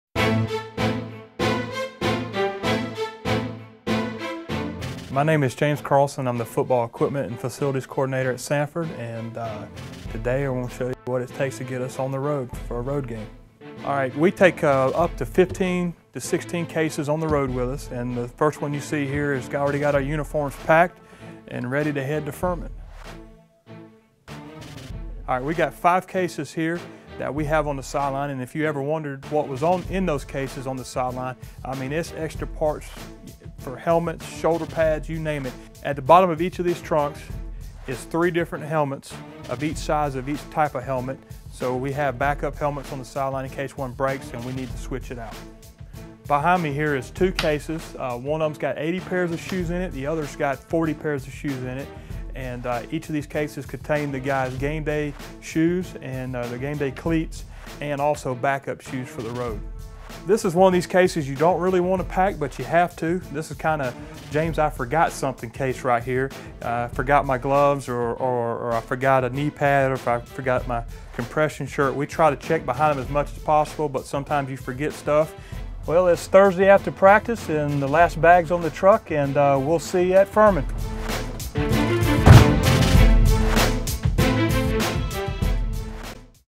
Behind-The-Scenes with James Carlson
Samford Equipment Manager, James Carlson, shows what it's like to be the equipment manager for the football team.